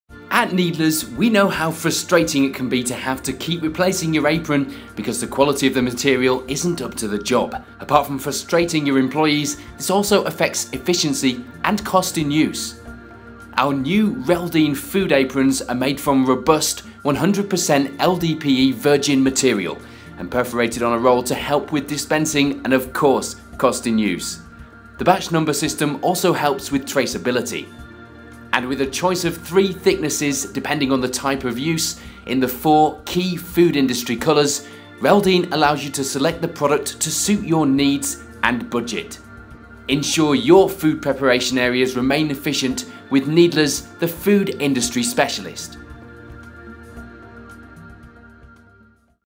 Reldeen food industry aprons
The food industry has had enough of poor quality disposable aprons that need to be replaced every few minutes.

So has Needlers, the food industry specialist. So we've launched the new Reldeen food aprons to help you keep your employees comfortable, and your food areas efficient.

For more food industry updates and innovations follow the food industry specialist on Twitter - @needlersPPE.

This product is available from December 2013.